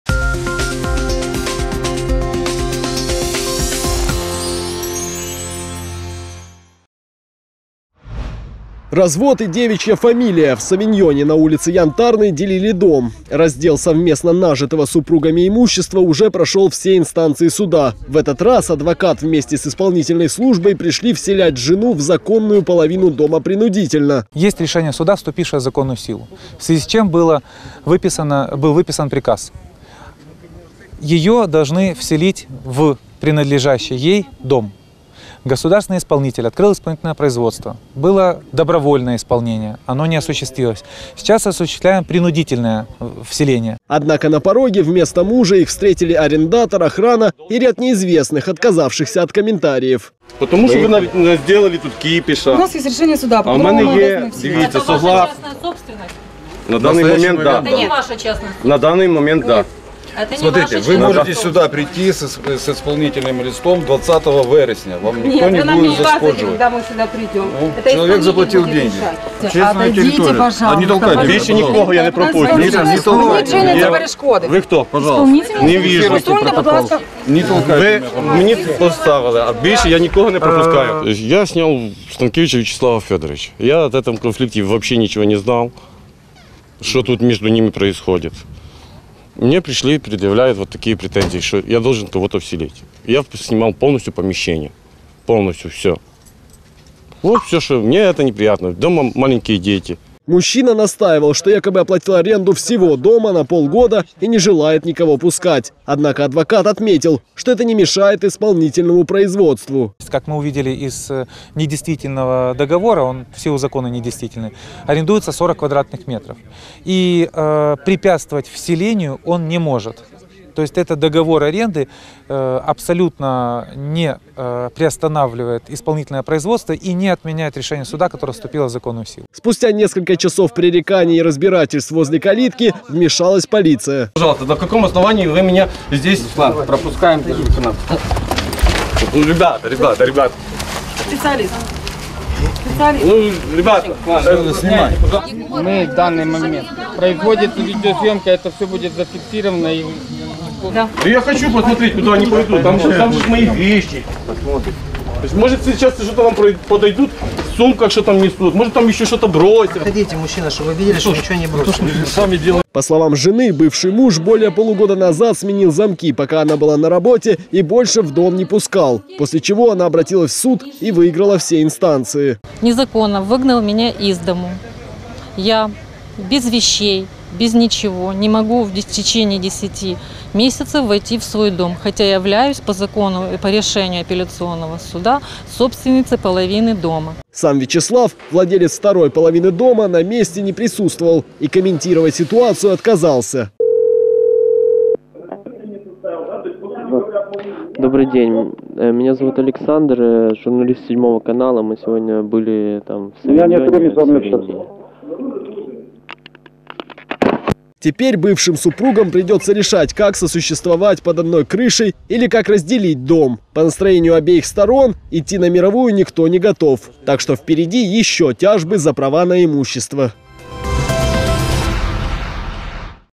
Раздел имущества бывшими супругами
Развод и девичья фамилия – в Совиньоне делили дом. Раздел совместно нажитого супругами имущества уже прошел все инстанции суда. В этот раз адвокат вместе с представителями исполнительной службой пришел вселять жену в законную половину дома принудительно.